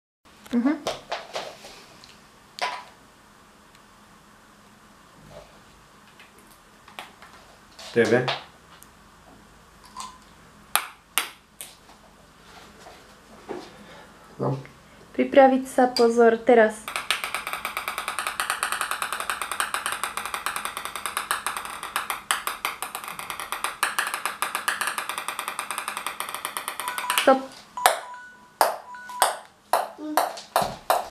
Most Table Tennis Ball Bounces on the TV remote control 10 Seconds And Wearing Sunglasses
10 Seconds 95 Bounces Tennis Ball  the TV remote control